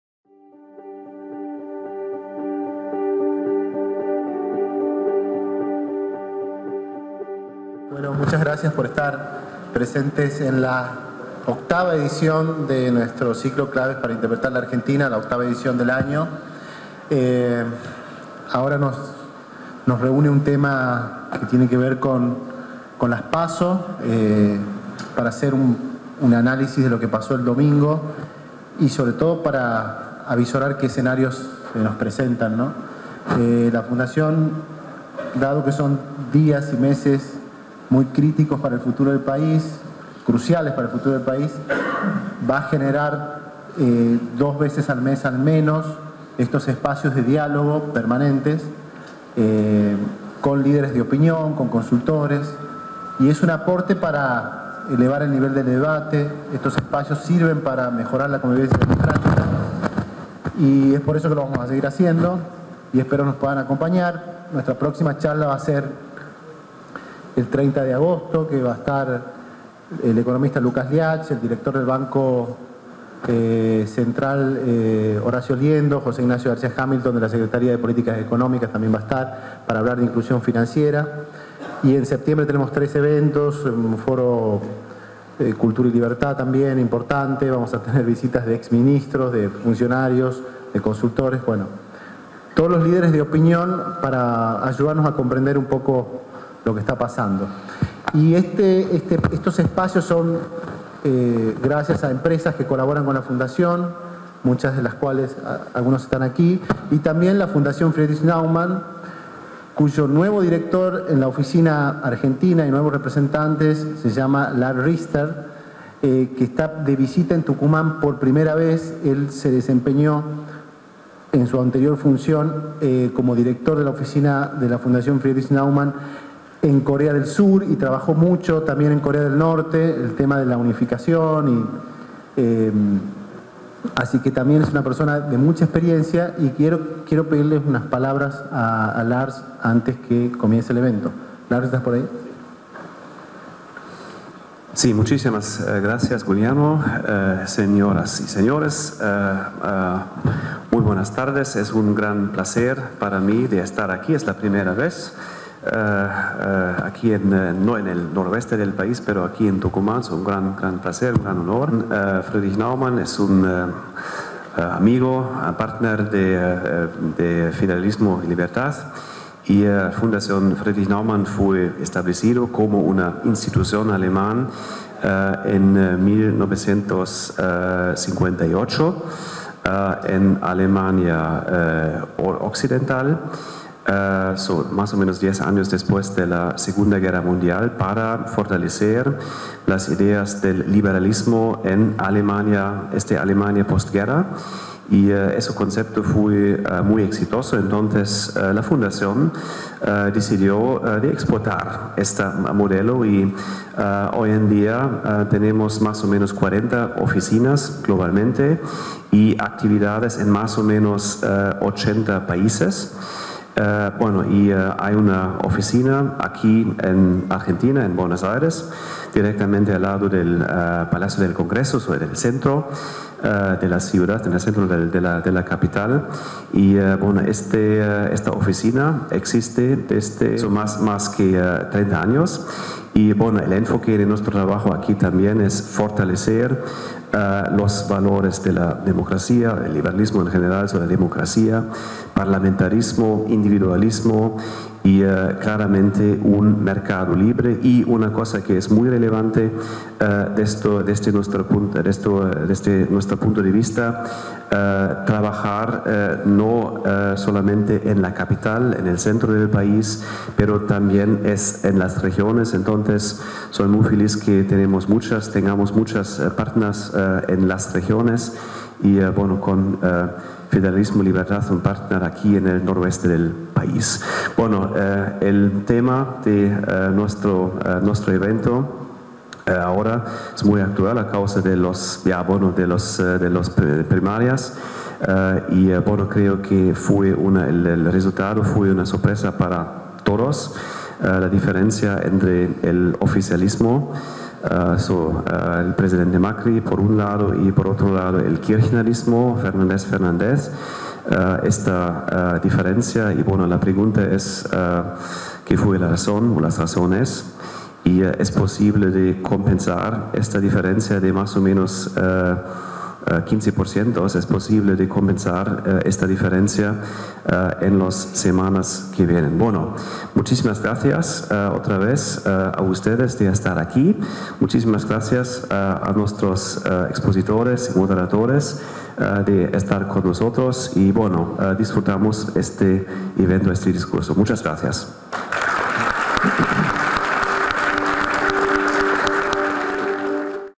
Palabras del Dr. Lars-André Richter en la apertura de la conferencia “Análisis Post PASO”
En el marco del ciclo de conferencias «Claves para interpretar la Argentina», este miércoles 14 de agosto tuvo lugar la conferencia “Análisis Post PASO”, a cargo del consultor Jorge Giacobbe y el periodista Fernando Stanich.

Las palabras de apertura estuvieron a cargo del Dr. Lars-André Richter, Representante de la Fundación Friedrich Naumann para la Libertad (FNF), quien visitó Tucumán por primera vez.

Ayudanos a seguir trabajando para construir una sociedad más libre y democrática